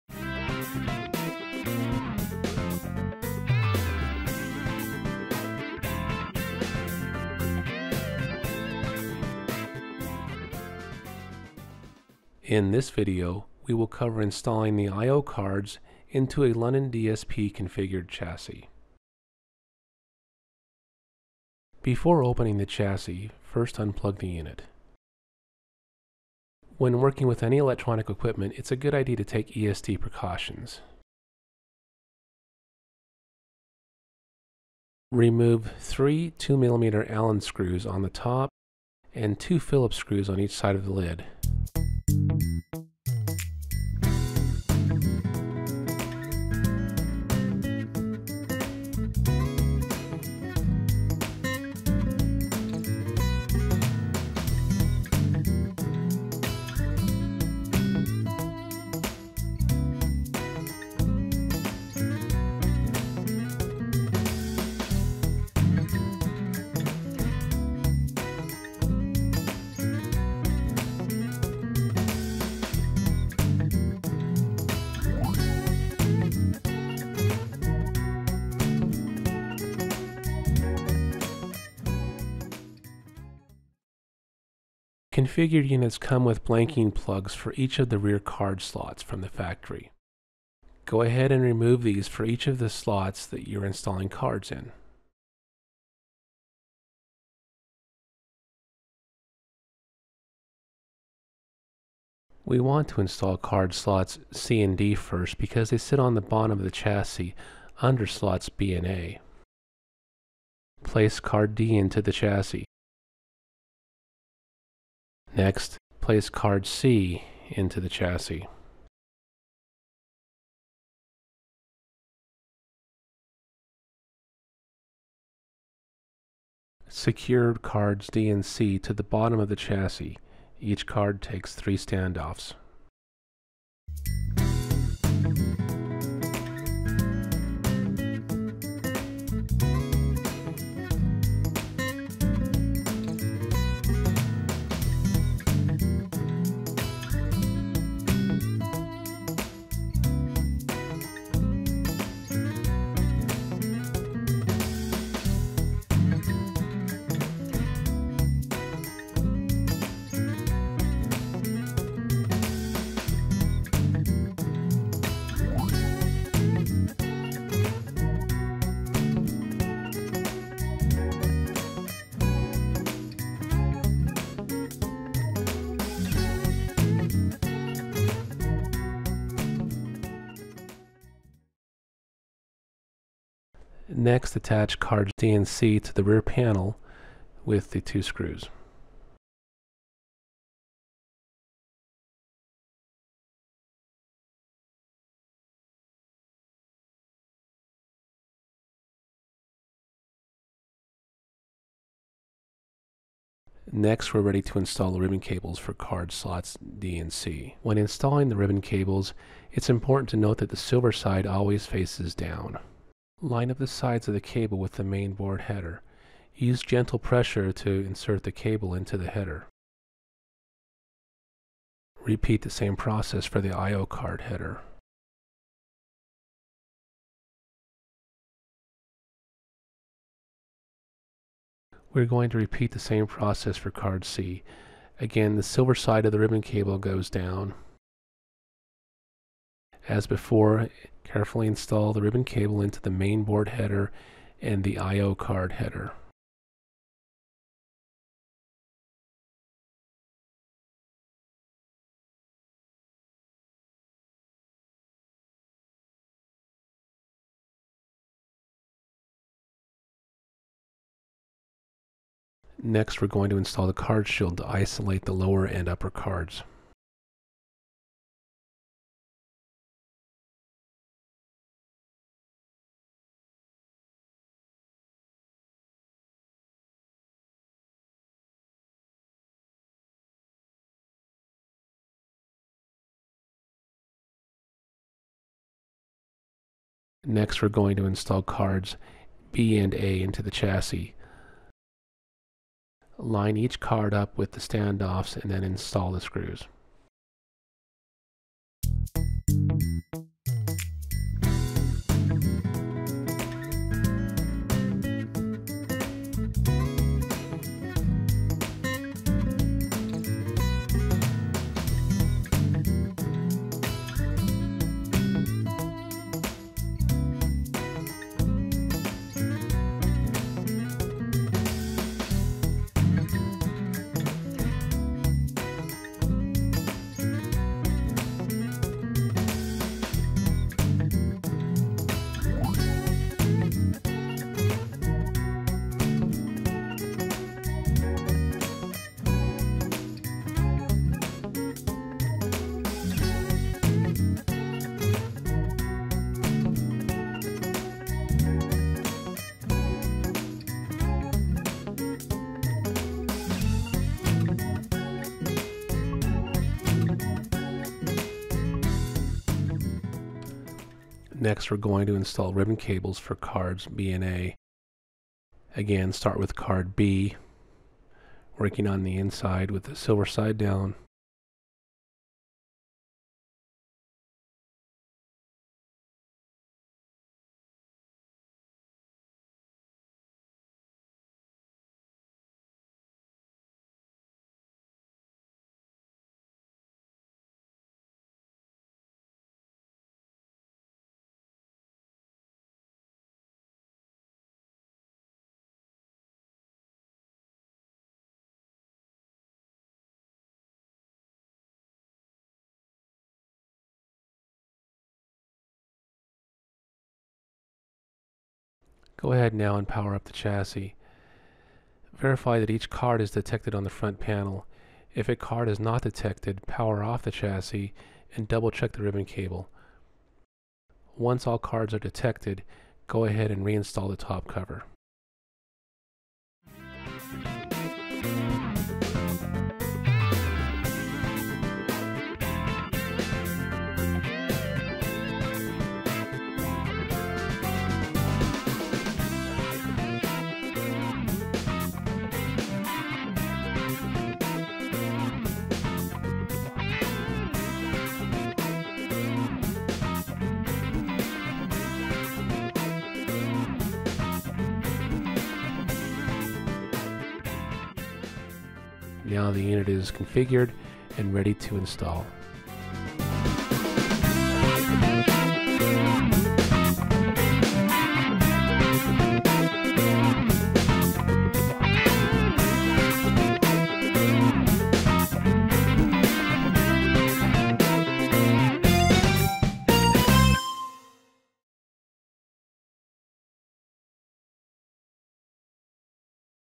BSS | Installing I/O Cards with Soundweb London Hardware
In this video, we demonstrate the steps for removing the lid and installing I/O cards in a BSS chassis. If you have never done either of these things, this video is “recommended” viewing. Topics include: removing and replacing screws and the chassis lid, installing and securing cards, and connecting internal ribbon cables.

Special thanks to: Bryan DeWeese and John Clark of HARMAN Professional Technical Support for their development of this video.